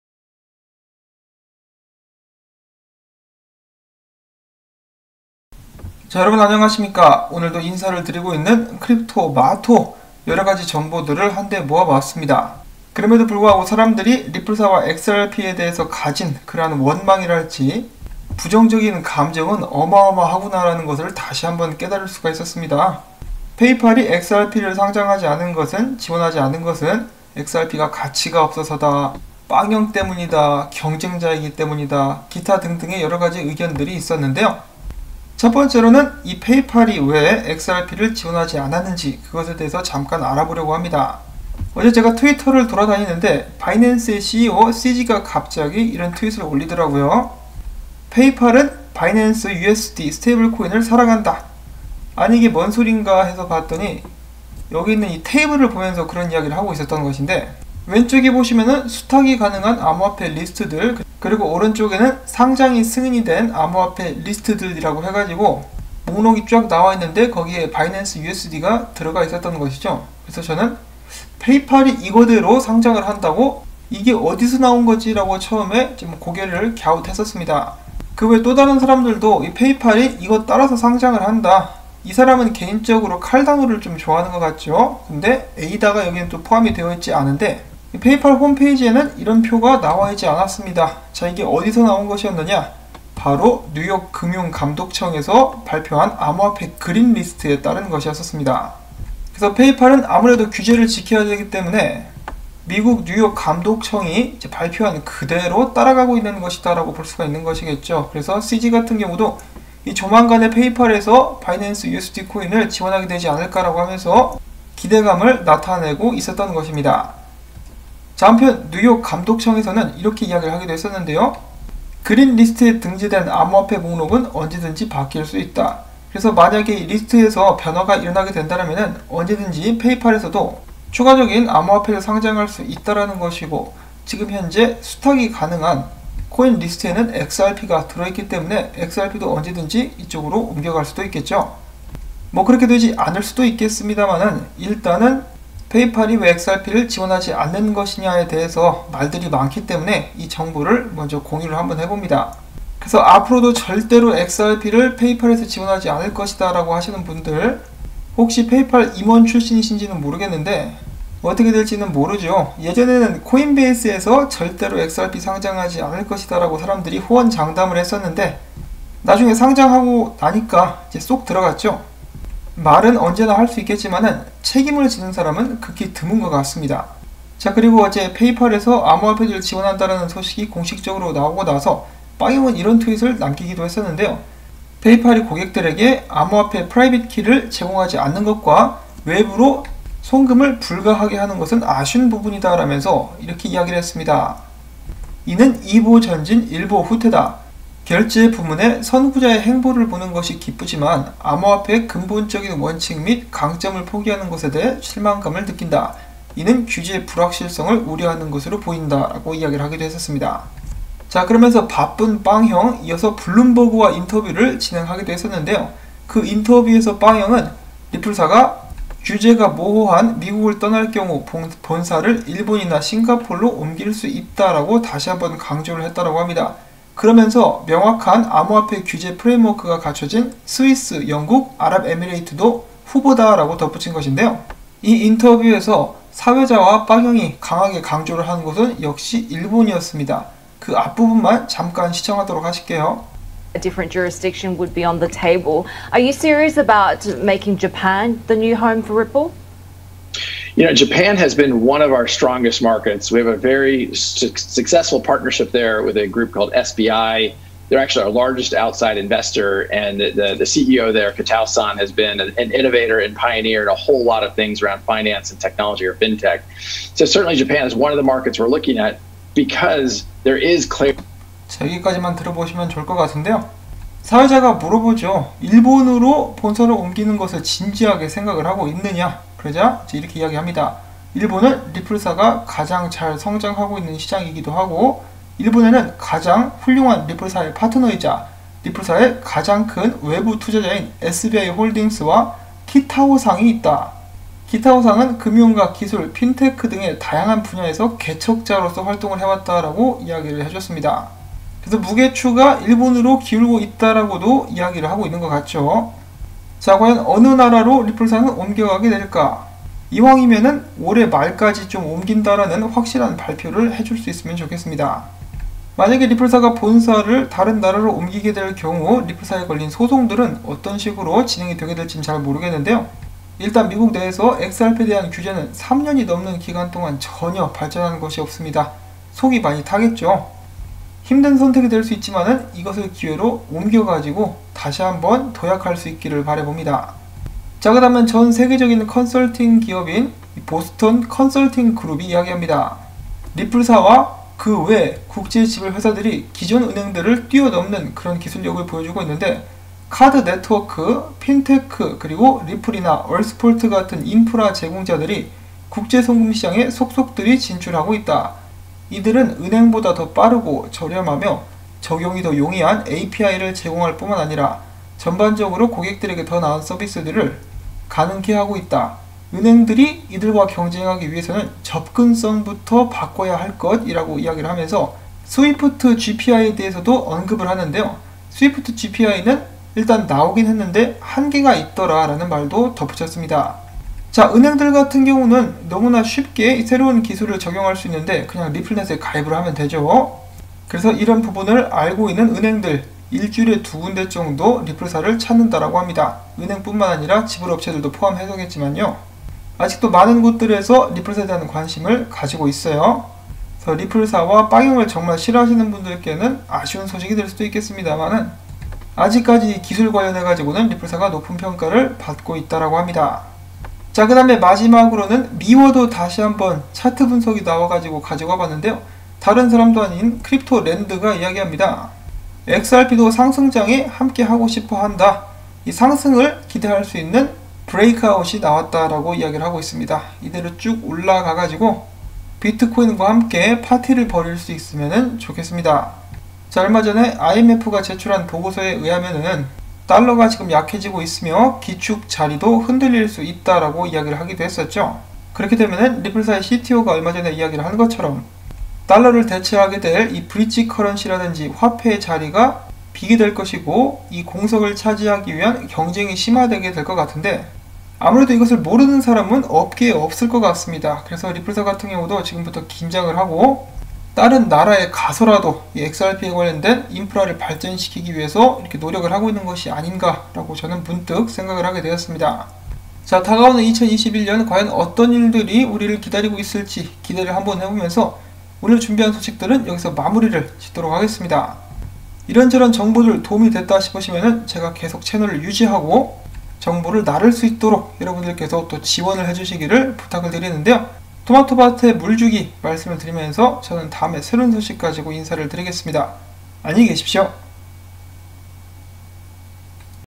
리플/XRP) 리플사 일본으로 간다? 보스턴 컨설팅그룹 리플사 평가, 페이팔이 XRP 제외한 진짜 이유,
*내 자산을 안전히! 한글 디센트 하드웨어 월렛(할인 링크)단일상품: